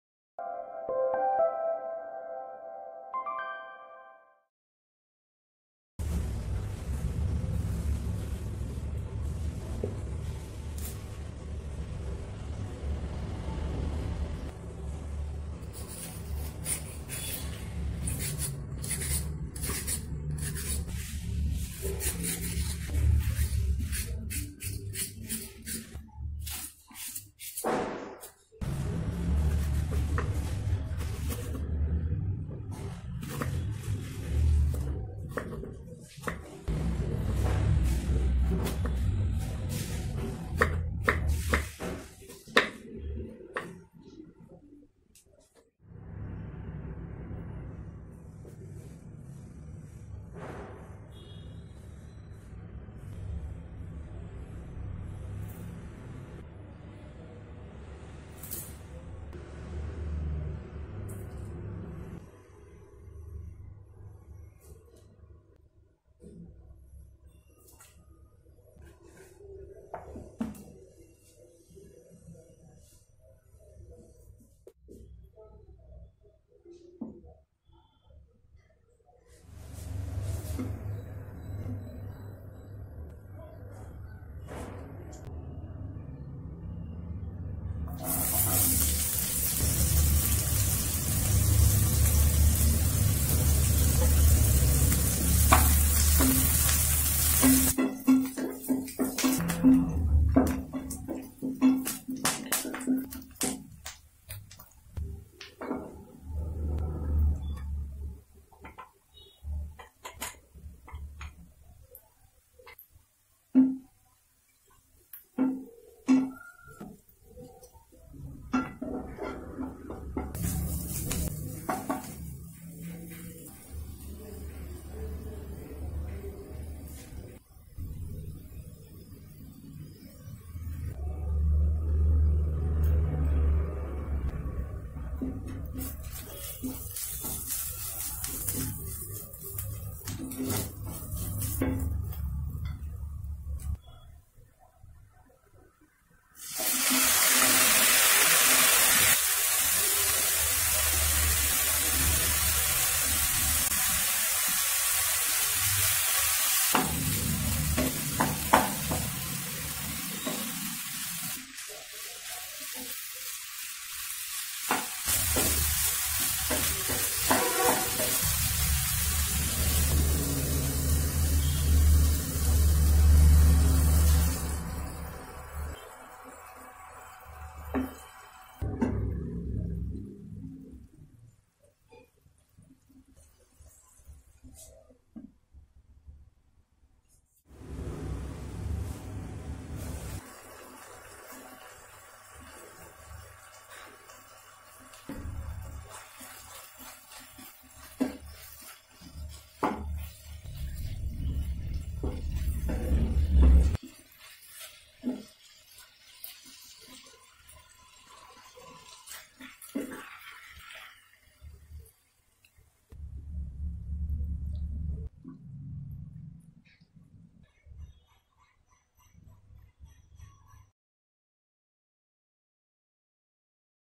Have Ever Tried This Recipe With Raw Banana | Best Side Dish For Roti & Rice | Raw Banana Fry
INGREDIENTS REQUIRED :
Masala Paste Making :

FRESH COCONUT(grated) - ½ cup, 50g
FENNEL SEEDS(sompu) - ½ tsp
PEPPER SEEDS(meṇasu) - ½ tsp
Banana Fry Making :

RAW BANANA - 6 medium size
WATER - 1 cup, 100ml
GARAM MASALA - ¼ tsp